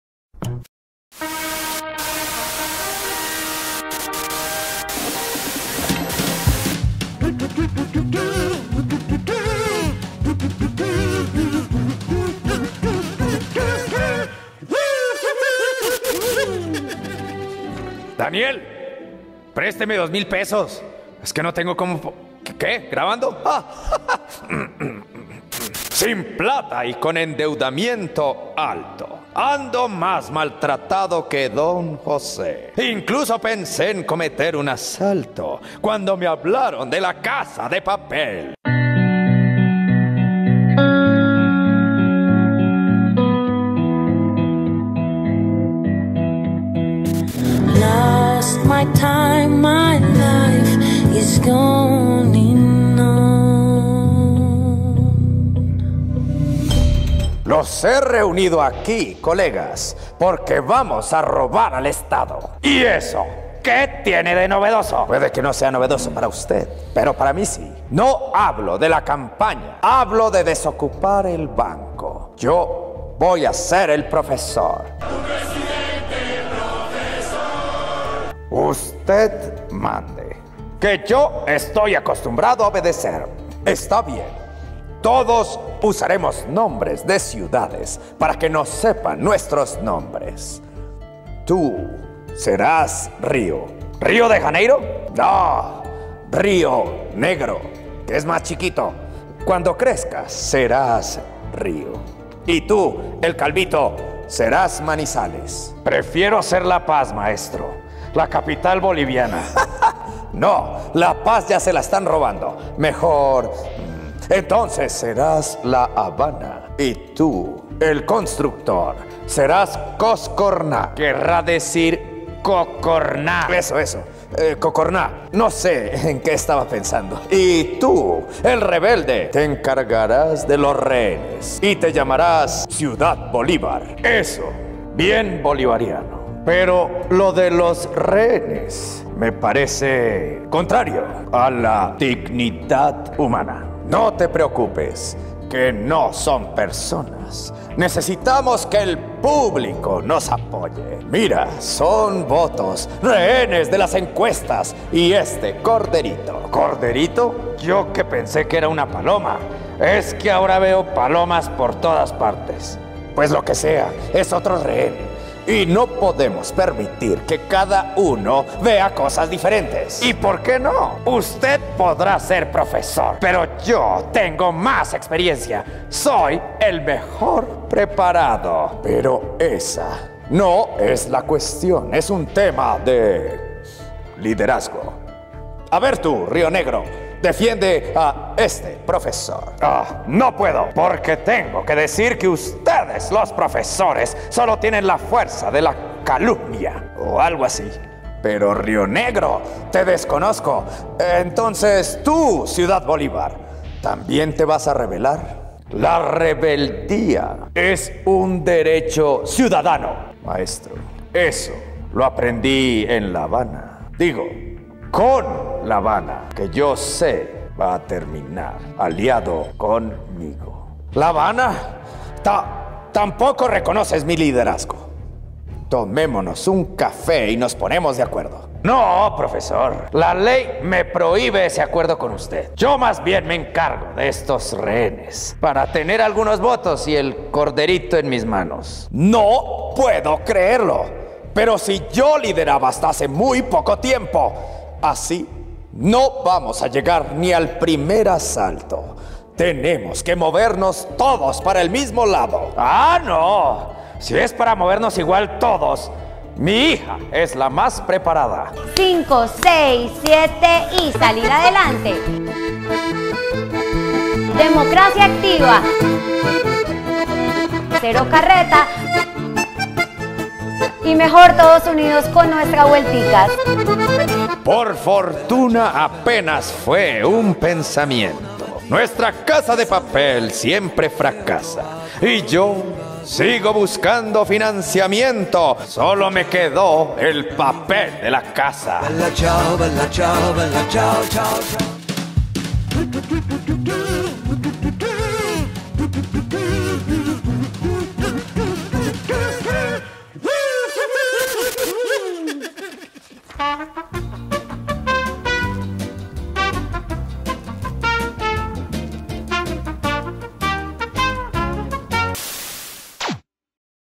Candidato se inspira en la Casa de Papel para conseguir dinero para la campaña I CEET
Cándido, el candidato-títere, organiza un plan con sus colegas para tener más plata. ¡Empieza el titirercado!